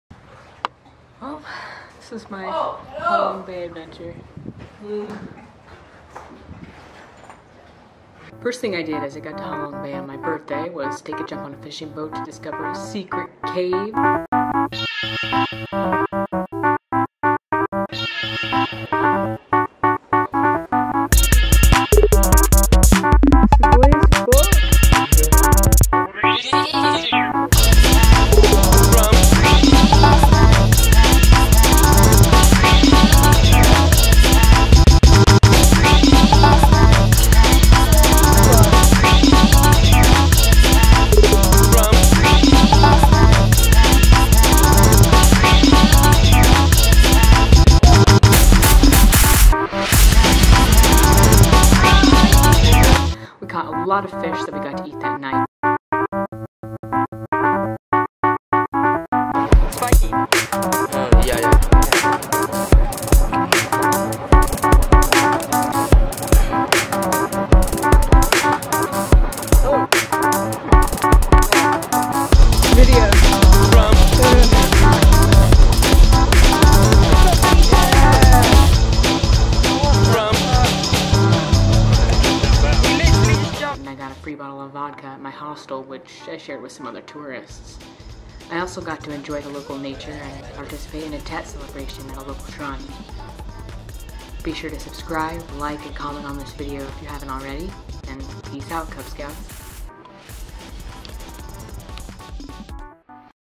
Halong Bay Adventures!!
It was my birthday right as I arrived in Halong Bay, and though it was a bit of a trek to get to my hostel, I was welcomed warmly and here's some of the shit I ended up doing!